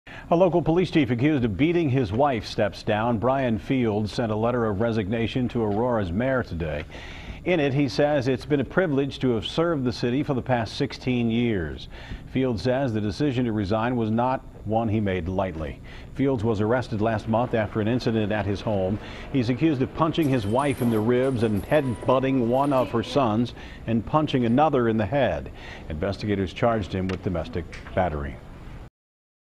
Resignation Letter from Former Aurora Chief of Police
AURORA, Ind. (WKRC) -- Aurora's former Chief of Police has released his letter of resignation.The resignation goes into effect Friday, January 10th citing the, need to take time to concentrate on my family.Fields' decision comes after his arrest in December on charges of domestic battery and battery from an altercation involving Fields, his wife, and two teenage children.CLICK HERE to read the entire resignation letter.